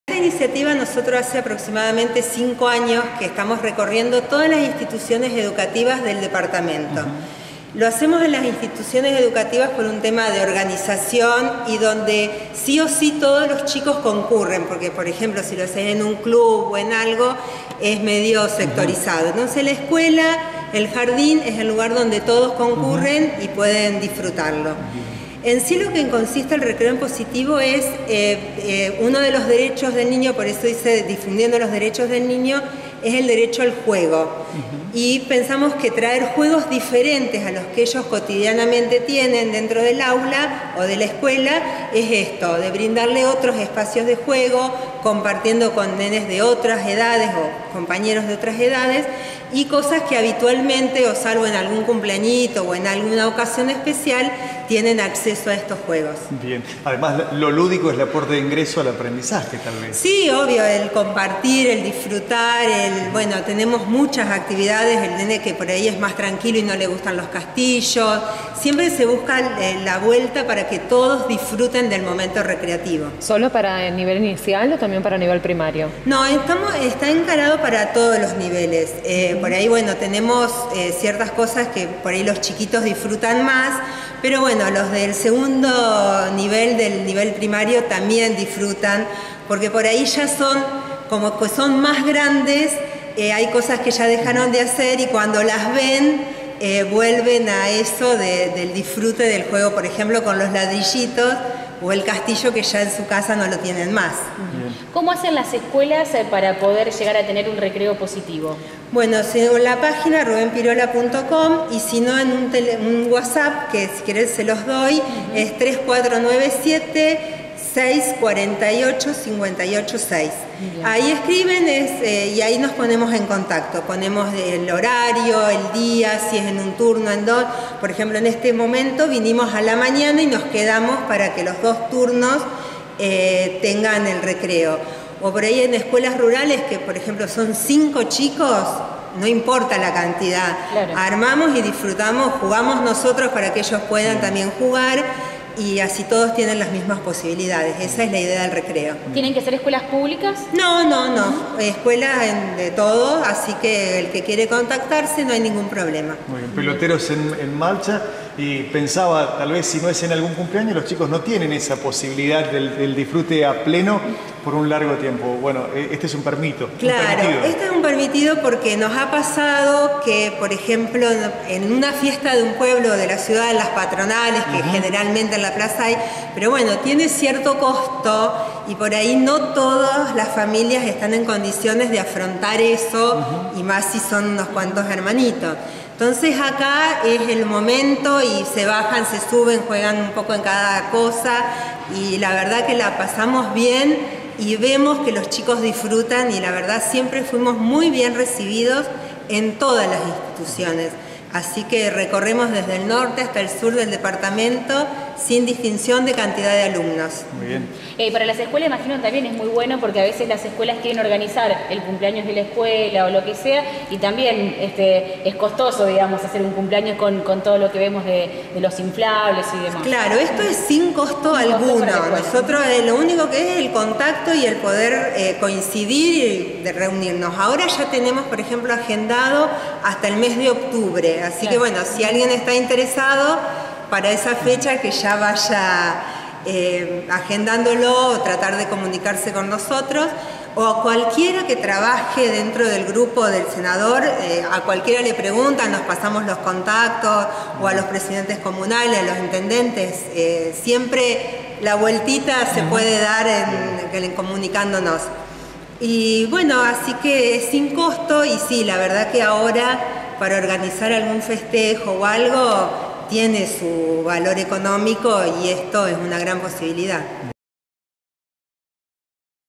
PROGRAMA “UN RECREO EN POSITIVO” - REALIZARON ACTIVIDADES EN EL JARDÍN Nº 254
IMPULSADO POR EL SENADOR PIROLA